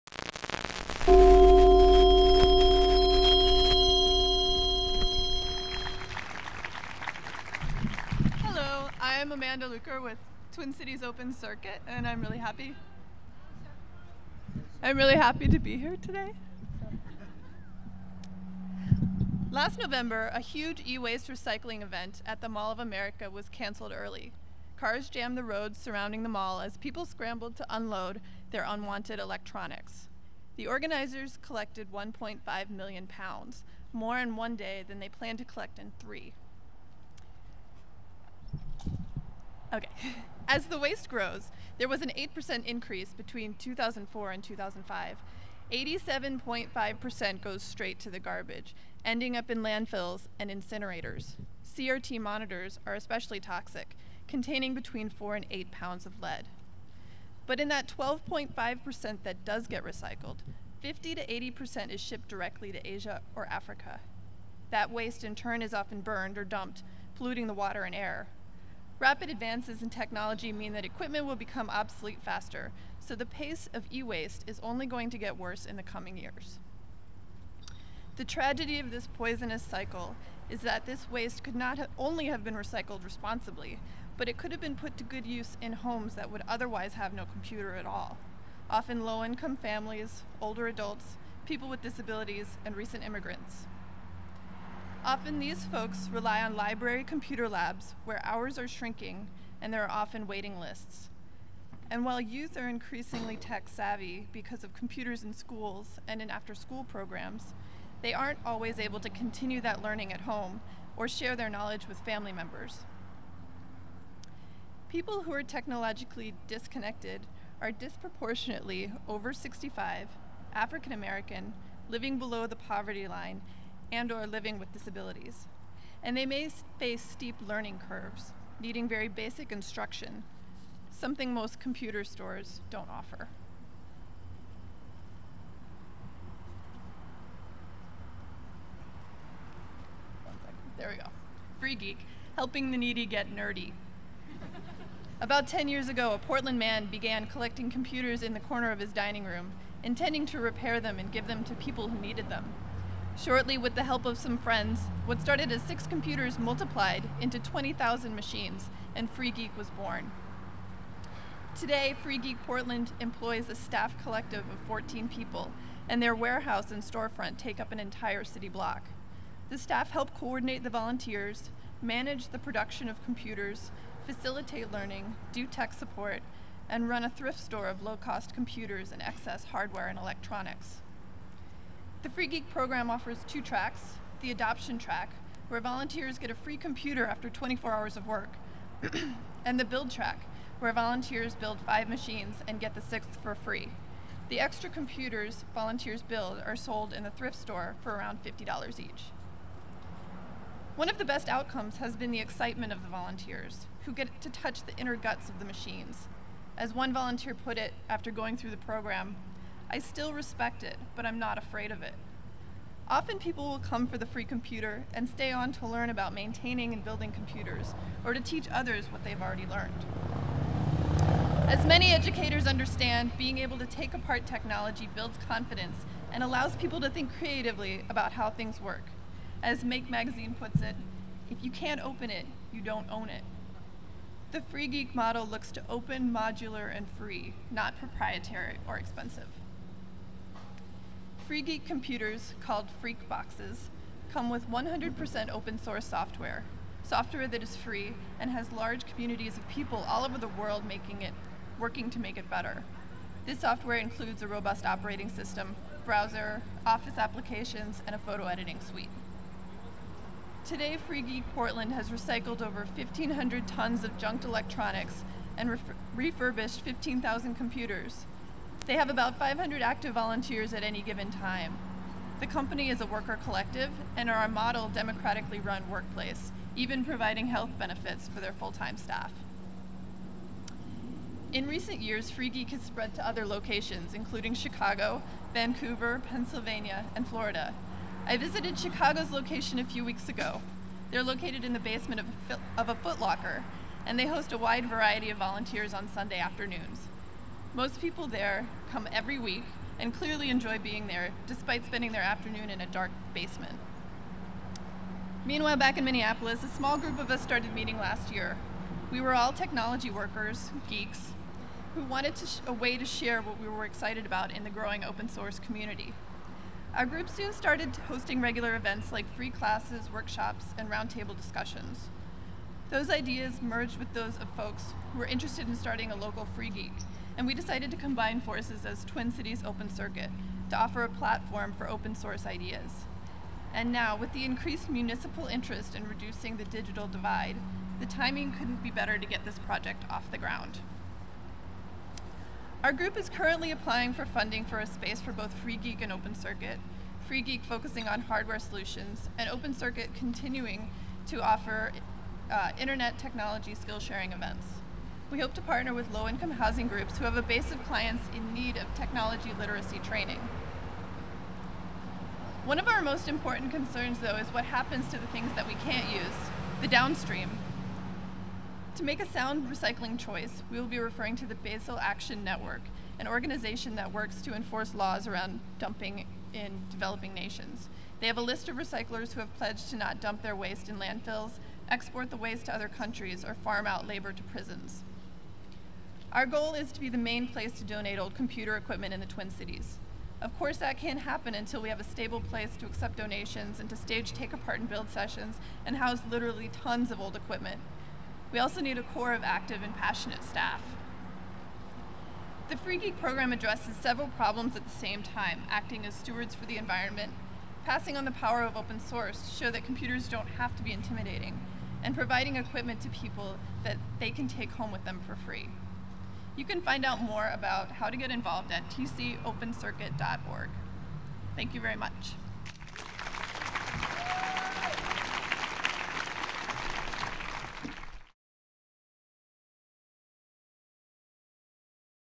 Twin Cities Open Circuit on closing the technology gap
Amanda Luker - Twin Cities Open Circuit

Twin Cities Open Circuit is a technology skill-sharing organization founded in 2007 with the mission of spreading the free and open source software movement. Free Geek is their newest program designed to help close the technology gap in the Twin Cities. Currently in the works, it will offer computers, education, internet access and job skills training to those in need in exchange for community service. Participants will help recycle and refurbish used technology, learning valuable skills along the way. The program is following in the successful footsteps of Free Geek chapters in Portland, Chicago, Vancouver and other cities across the nation.

Recorded on August 30th, 2008 at Solutions Volume 3 in Minneapolis, Minnesota.